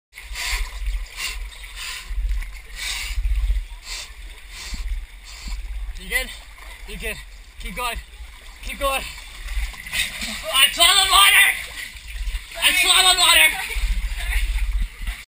Whistler Tough Mudder 2013 - Boa Constricter: What NOT to do!
My sister conquered her fear of small spaces.  With me guiding her in the back, i thought i'd be safe... I thought wrong...

Boa Constrictor:
If you don't like small spaces, this obstacle will be a challenge for you. Crawl through a series of pipes that force you downhill into some freezing mud, then a slippery uphill climb to the other side. Your legs will be useless in the narrow confines of the Boa, so use your arms to pull yourself through this obstacle. There really is light at the end of the tunnel. Check out photos and video of Boa Constrictor below.